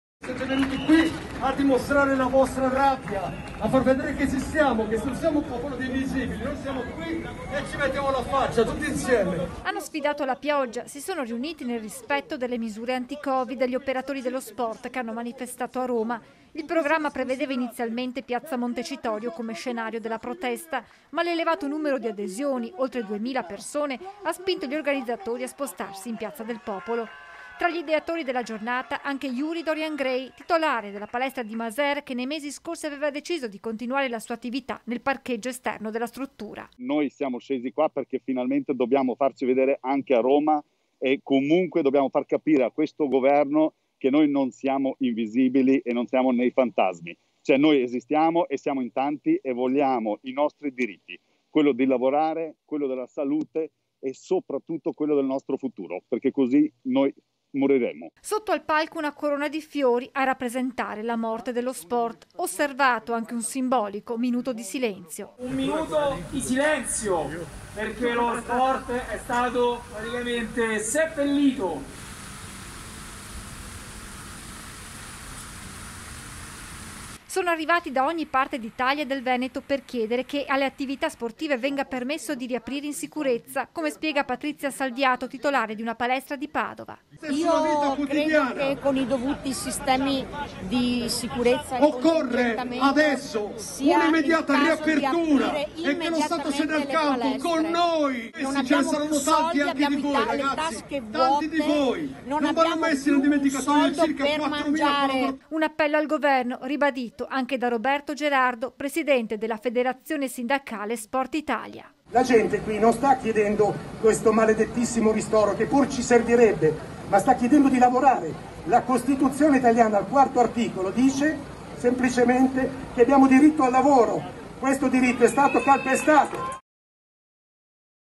IN PIAZZA DEL POPOLO LA PROTESTA DELLO SPORT: «VOGLIAMO TORNARE A LAVORARE» | 09/03/2021
A3 NEWS Veneto 09/03/2021 - ROMA - Nel pomeriggio a Roma è andata in scena la protesta del mondo dello sport. Tra gli organizzatori Yuri Dorian Gray , titolare di una palestra di Maser || Hanno sfidato la pioggia, si sono riuniti nel rispetto del distanziamento anti covid gli operatori dello sport che hanno manifestato a Roma. Il programma prevedeva inizialmente piazza Montecitorio come scenario della protesta, ma l'elevato numero di adesioni, oltre 2mila persone ha spinto gli organizzatori a spostarsi in Piazza del Popolo. Tra gli ideatori della giornata anche Yuri Dorian Grey titolare della palestra di Maser che nei mesi scorsi aveva deciso di continuare la sua attività nel parcheggio della struttura. Sotto al palco una corona di fiori a simboleggiare la morte dello sport, osservato anche un simbolico minuto di silenzio. Sono arrivati da ogni parte d'Italia e del Veneto per chiedere che alle attività sportive venga permesso di riaprire in sicurezza - Intervistati YURI DORIAN GRAY (Titolare palestra), PATRIZIA SALVIATO (Titolare palestra) (Servizio di Francesca Bozza) - Segui Antenna Tre anche sul digitale terrestre! Visita il sito